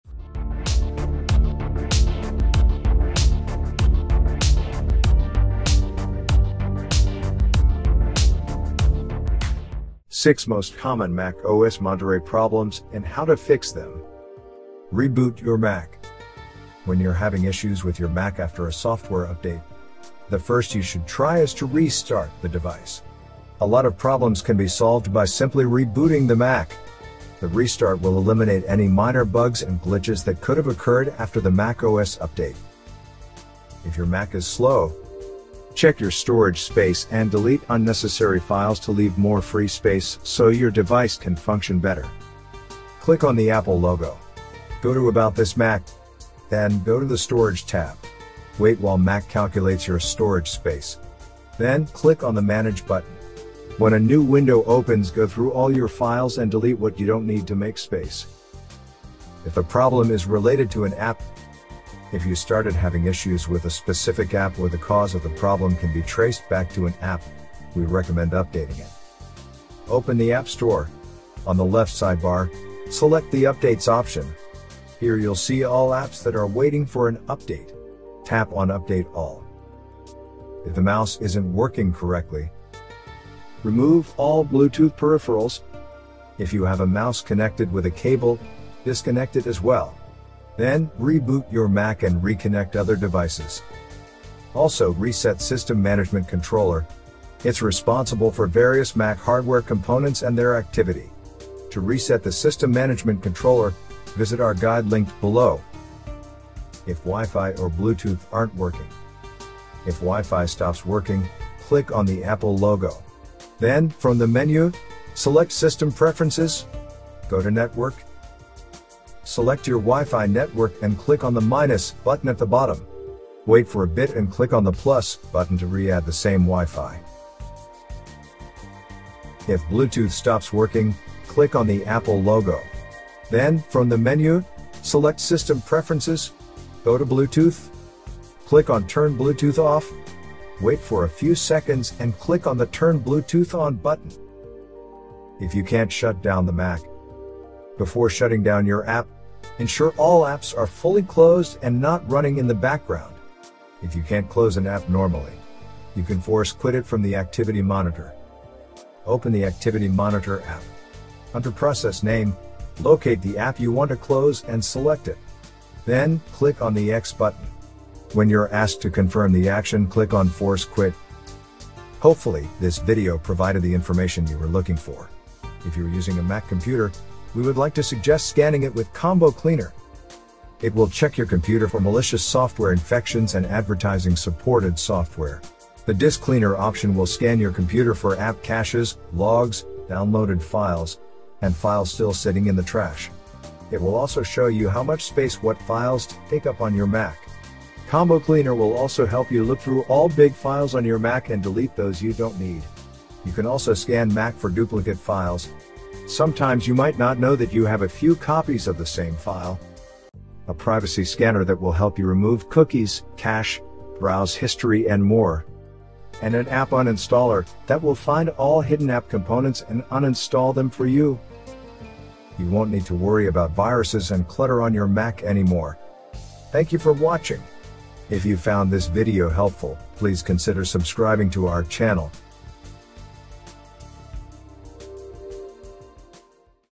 Having problems after the macOS Monterey installation? Here are the fixes
Video showing having problems after the macOS Monterey installation.